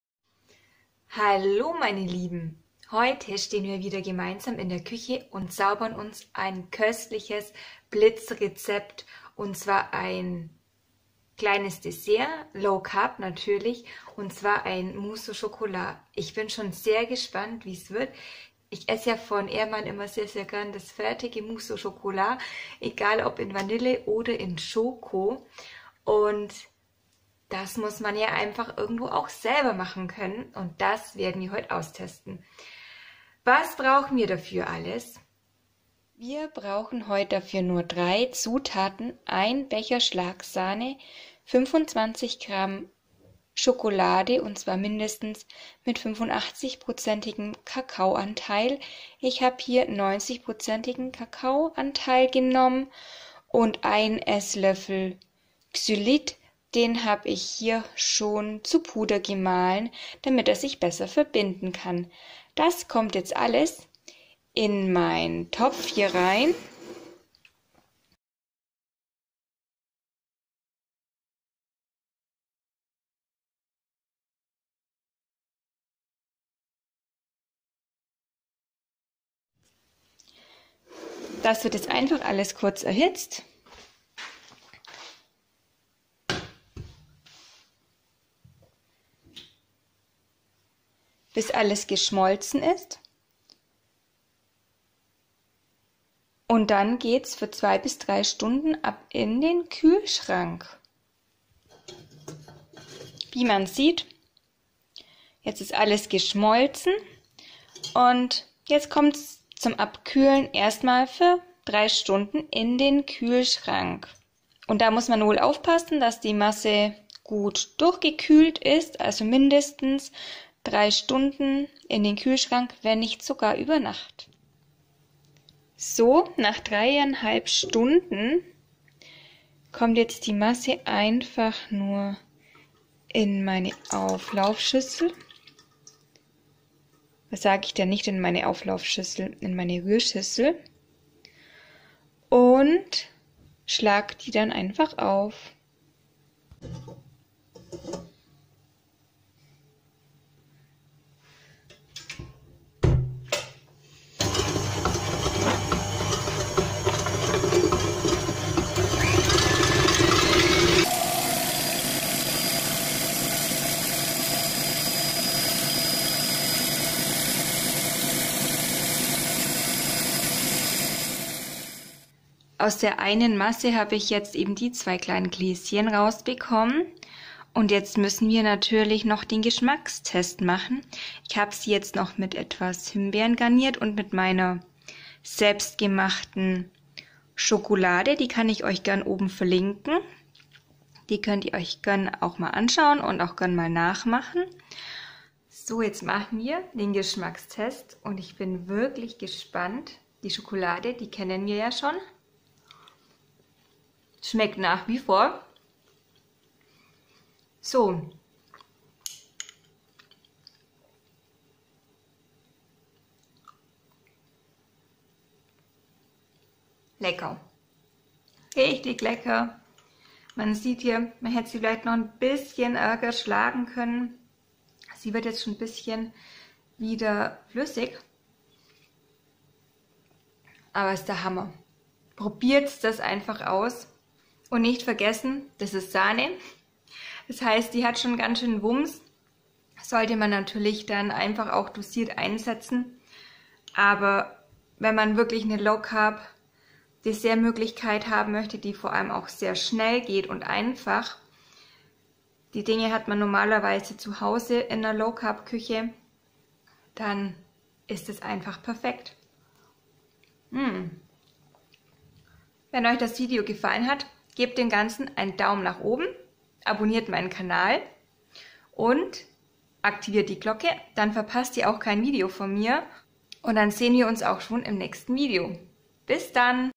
Mousse au Chocolat low carb - Blitzrezept
Zutaten:
1 Becher Schlagsahne
25 g Schokolade, mind. 85% Kakaoanteil
1 EL Xylit, zu Puder gemahlen

Meine Berater Nummer 61110

👉 Eigene Werbung, weil ich euch über meine Beraternummer zu Déesse-Produkten weiterleite und ich dafür eine Provision erhalte